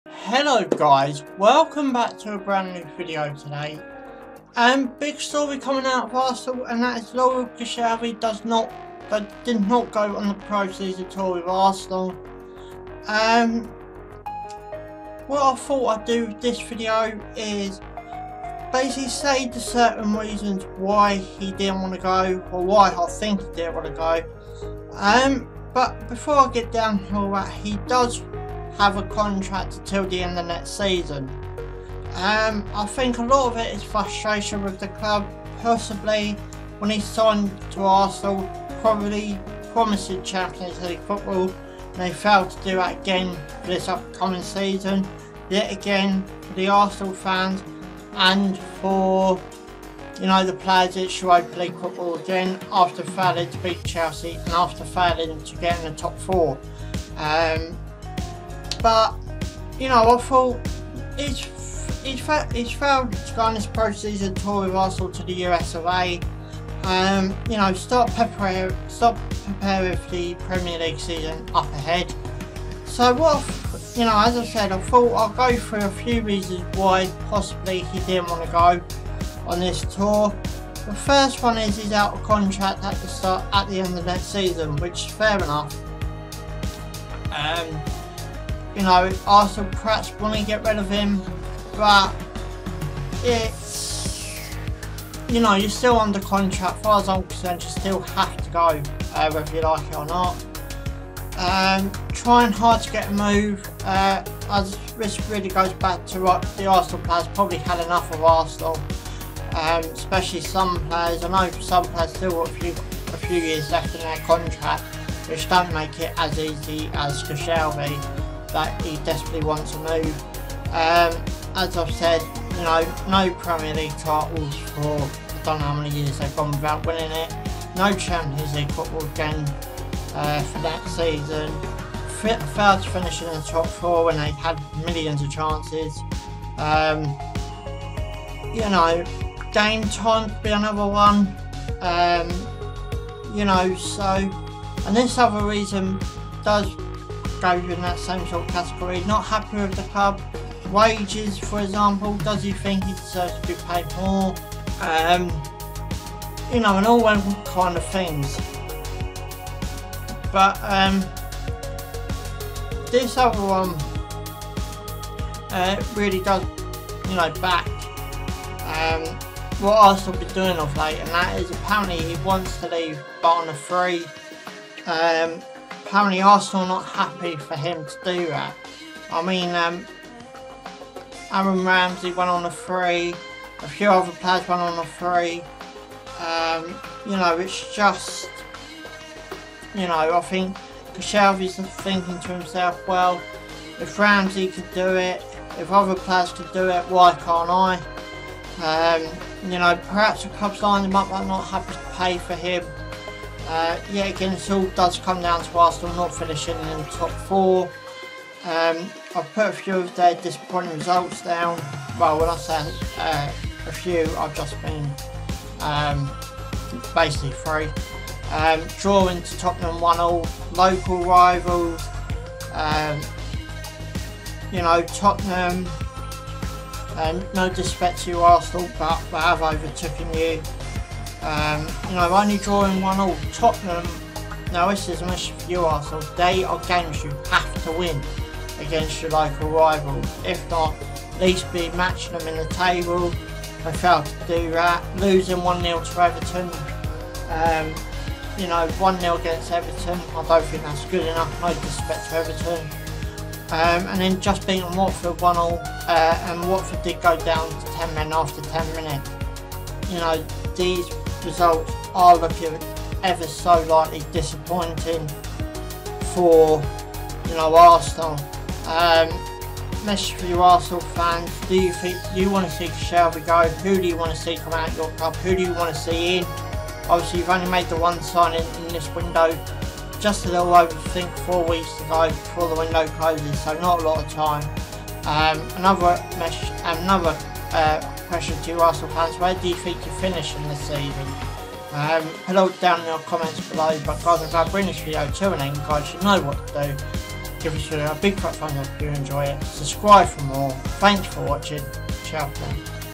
LAURENT KOSCIELNY DID NOT GO ON PRESEASON TOUR WITH ARSENAL 2019
why i do i think Laurent Koscielny did not want to go on preseason tour with arsenal